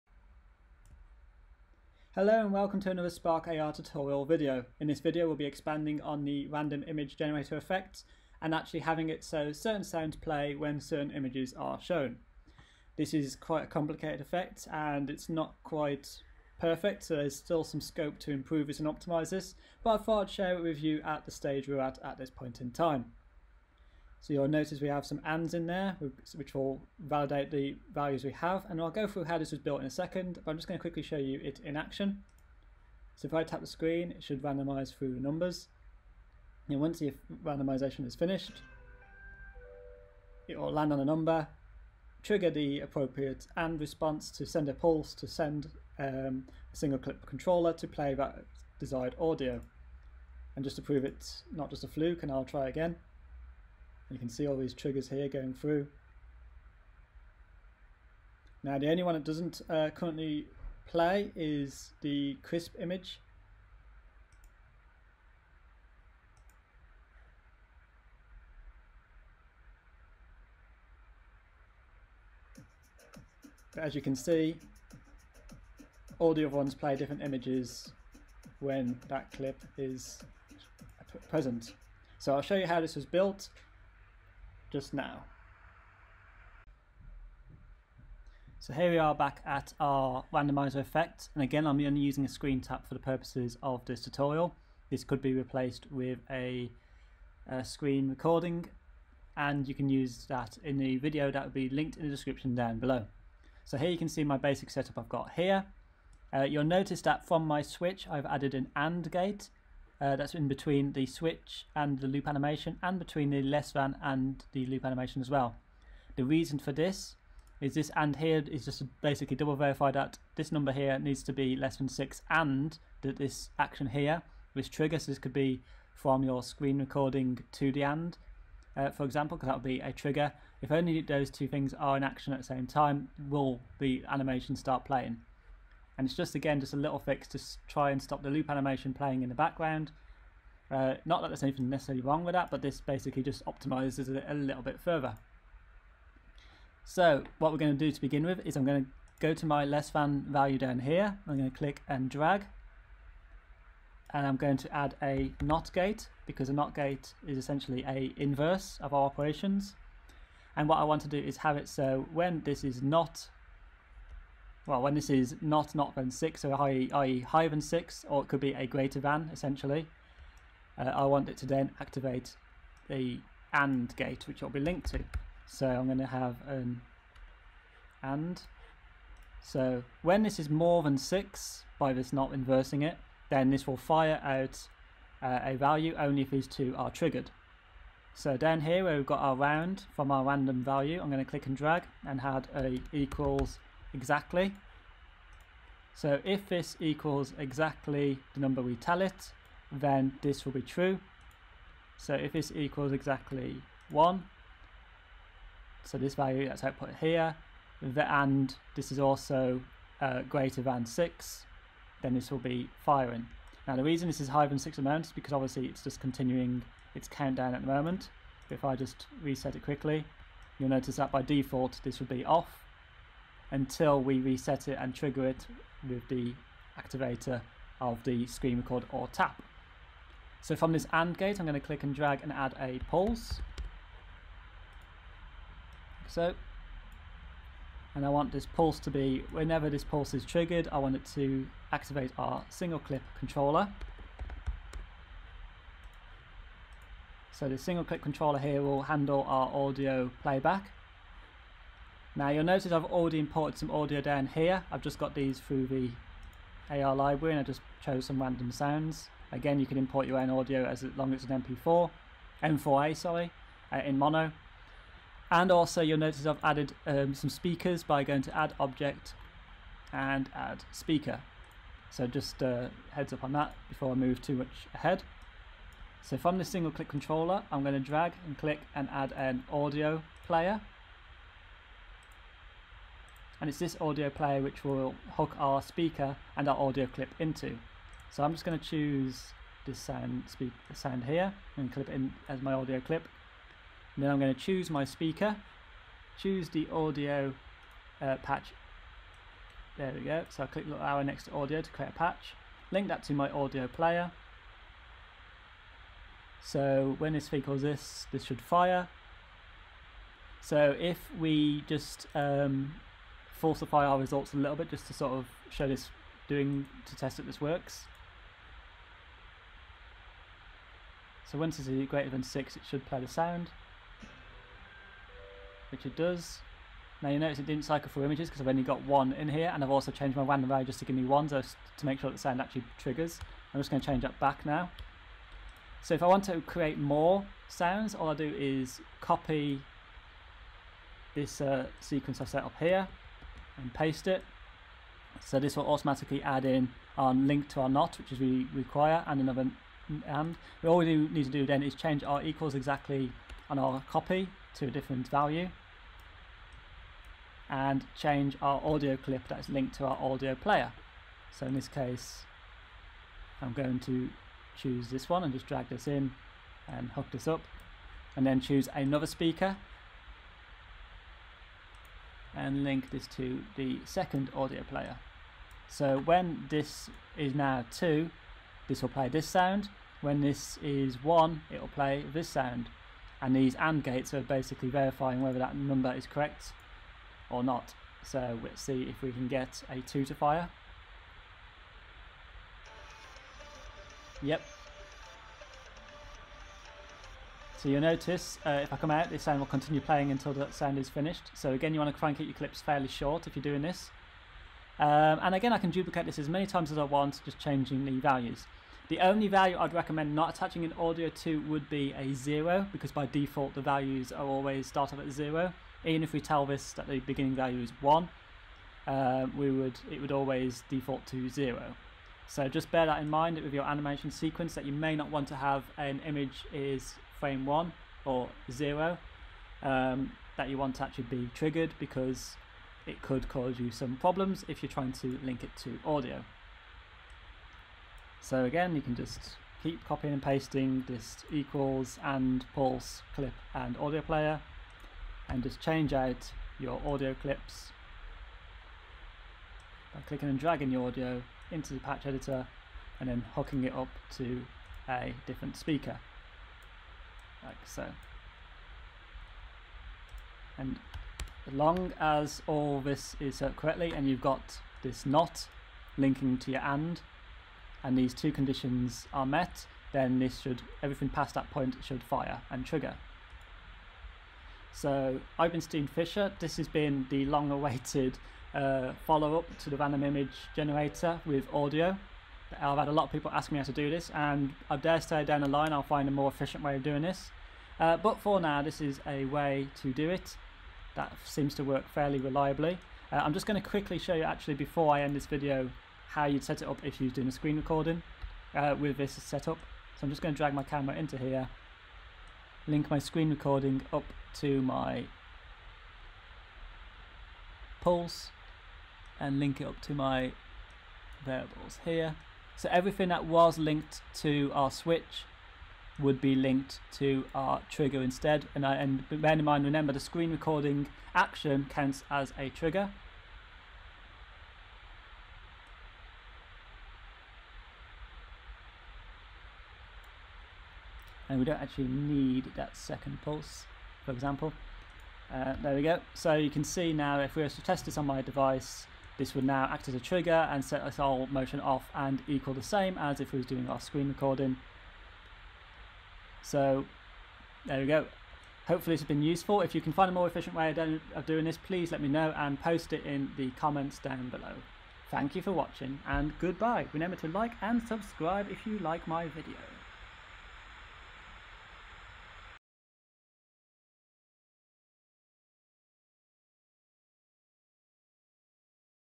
Triggering Sound with Value [Random Image Effect] | Spark AR Studio
In this video, we FINALLY answer one of the most asked questions from Random Image tutorial 'How do I trigger a set sound or action when value = x' In this video we find a solution and whilst it could be improved it is a working solution.

Please Note audio does not playback during recording only when recording is played back due to the way Spark AR deals with sound this is largely down to the microphone and spark stopping audio playback when recording audio from mic to stop potential feedback. Disable Microphone, Save project and relaunch after restarting PC/MAC then redeploy to phone to hear audio without mic.